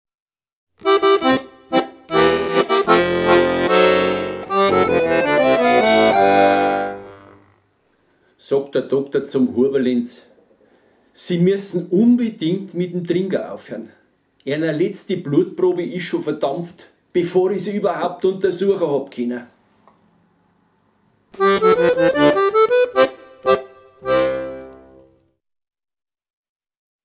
Die Blutprobe
Nichts verstanden? Kein Problem,
für alle Nichtbayern gibt es
nachfolgend eine Übersetzung:

Sagt der Arzt zu Lorenz Huber: "Sie müssen unbedingt mit dem Trinken aufhören. Ihre letzte Blutprobe ist bereits verdampft, bevor ich sie überhaupt untersuchen konnte."

Konrad Kneidlbeck: Rupert Frank
Akkordeon: Rupert Frank
Kamera: Barbara Lexa
Regie: Barbara Lexa und Rupert Frank